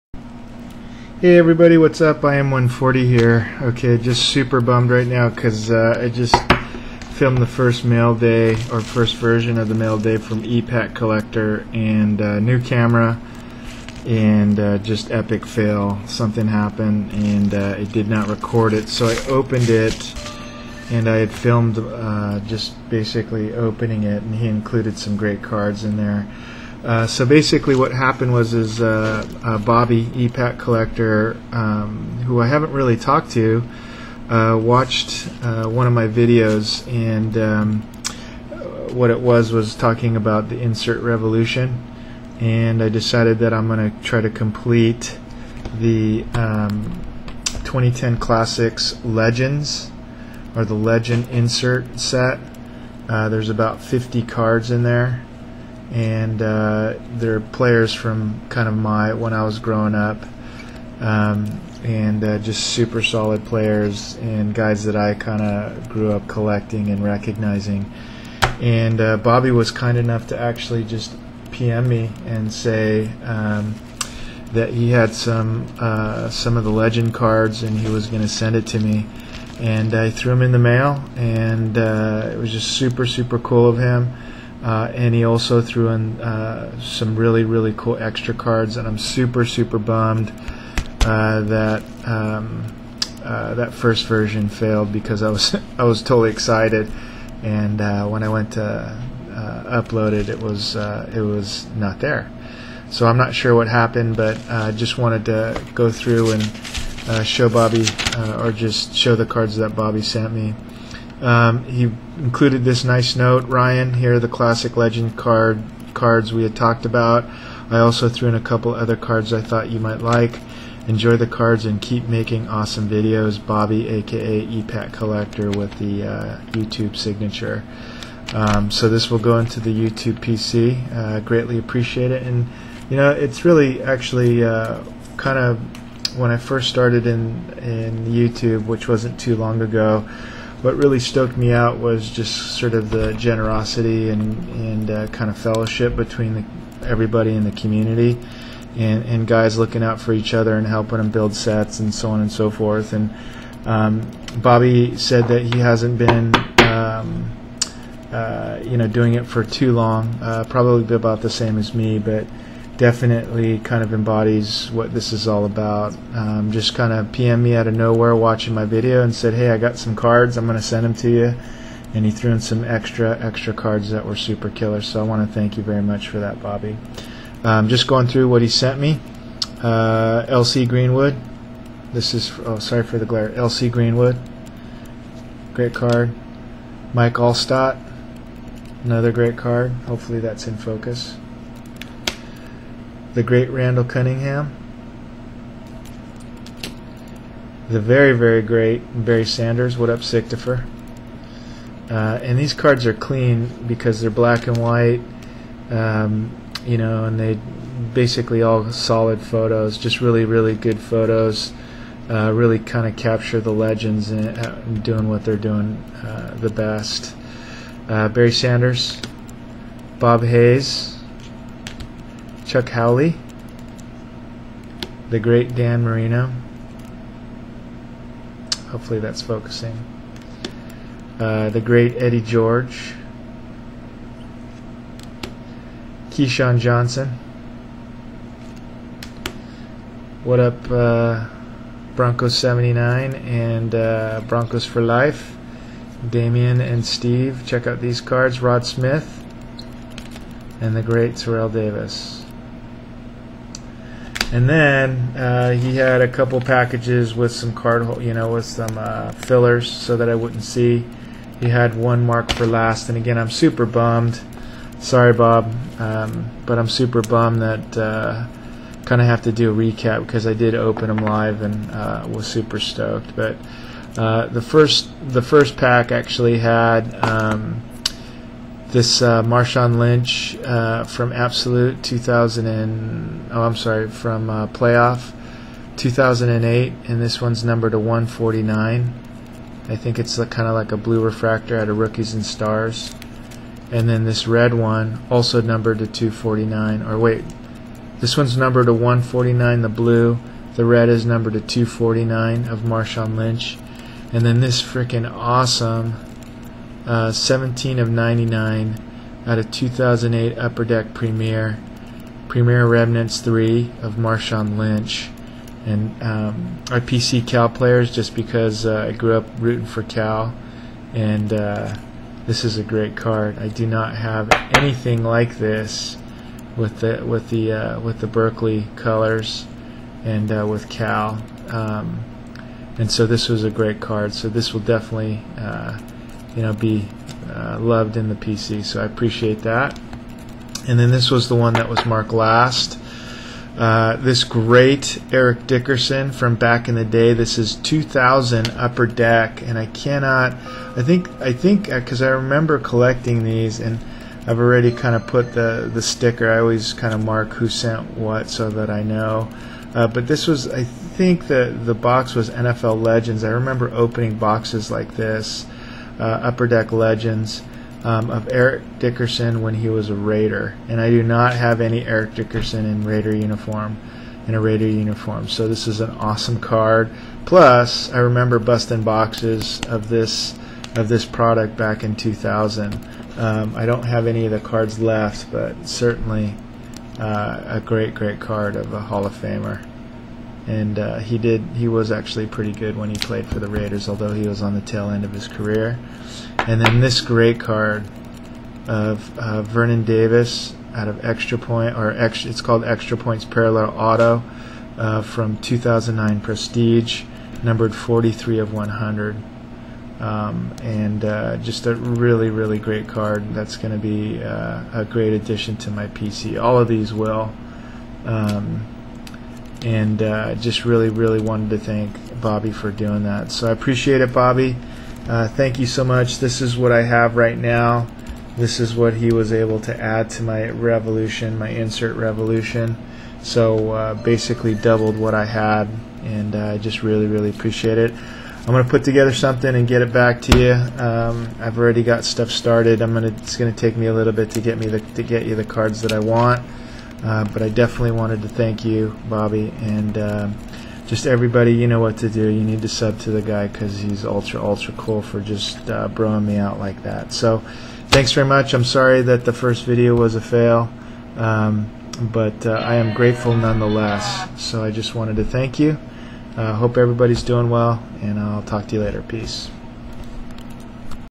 AWESOME JUST BECAUSE MAILDAY FROM EPATTCOLLECTOR!!! THANK YOU, BOBBY!
Received a pm last week from Bobby (aka epattcollector) in response to my 2010 Classics Legends insert revolution.  He sent me his Legends cards to help me with my set and also included some GREAT pc cards.

Unfortunately, I had to shoot the video twice.  FML.

Great guy and member of our community!